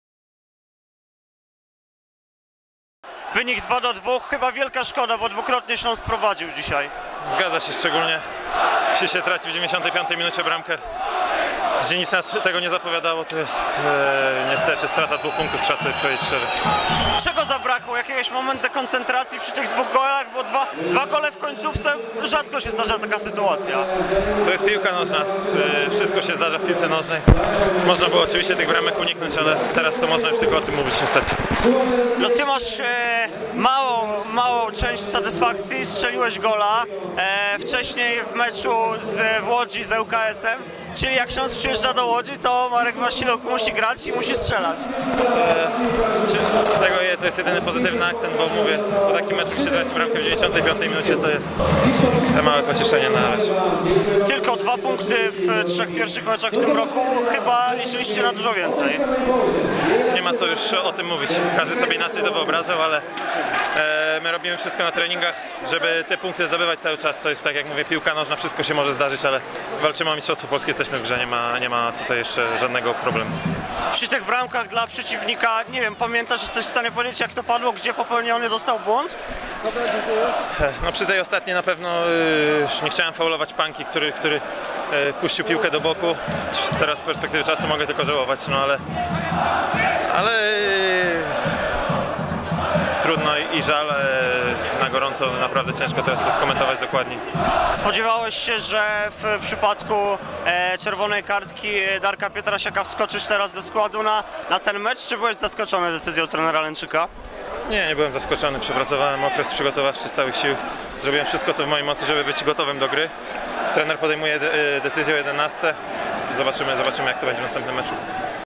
Marek Wasiluk po meczu Widzew - Śląsk (4.03.2012)
Wypowiedź Marka Wasiluka i otoczka spotkania Widzew - Śląsk z 4 marca 2012 roku.